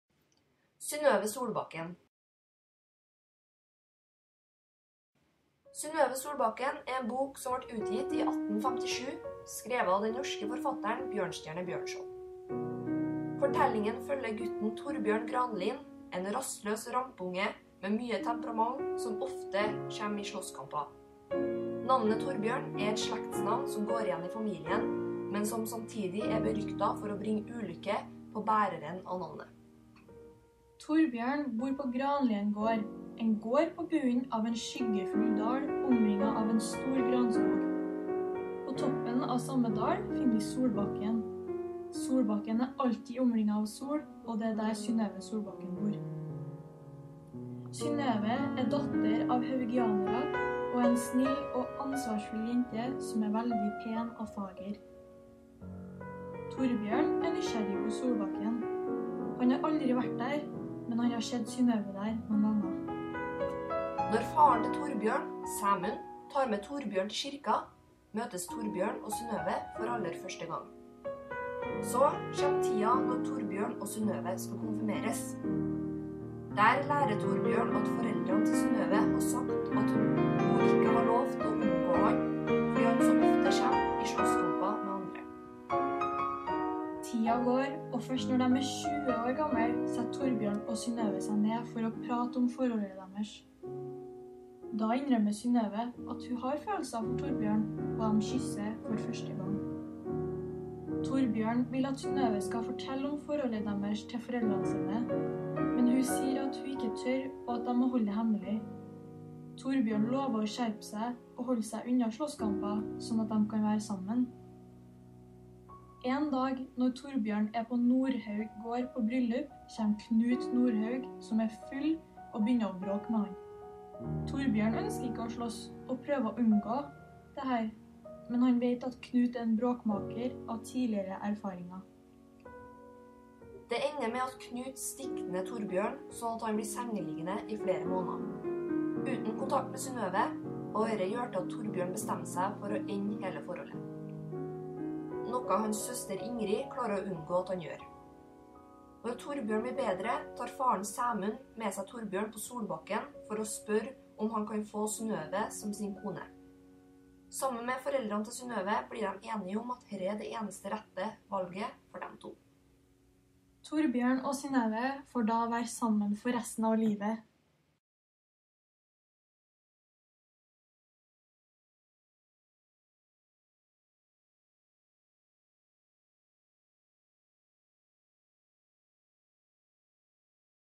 Synnøve Solbakken
Visualisering av gruppe 5: Vebjørn, Marthe, Julie, Silje, Duy Tan og John Christoffer.

Tegnet av: Vebjørn
Stemmer: Julie og Marthe